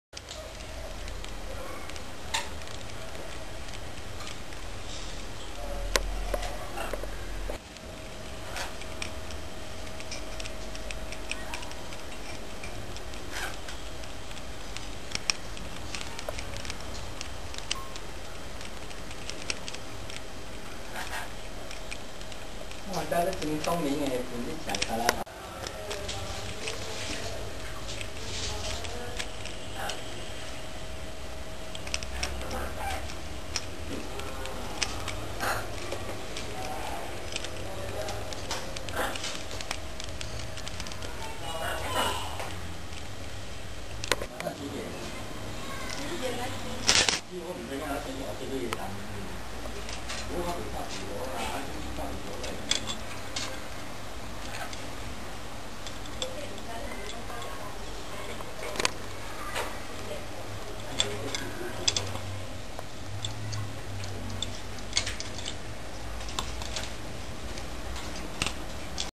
Xiao Feng (Little Wind) the Ducorp Cockatoo
Xiao Feng playing with some of his toys...